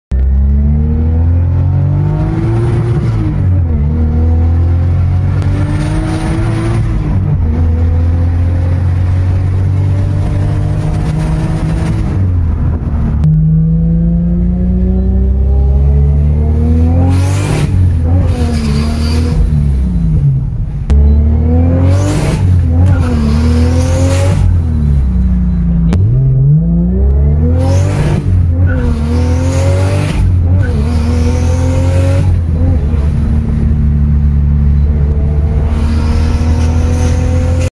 I was not expecting it to be this fast!! | 1JZ swapped BRZ
1JZ Engine swapped Subaru BRZ is ridiculous fast!!

Previous setup was tuned to 210kw (280hp).

Additional work done taking the car up to 250kw and 330hp at the wheels now!

It's a handful.

The 1jz BRZ was tinkered with for more power! New high flowing fuel pump added and upgraded waste gate spring. Then the car was retined for more boost.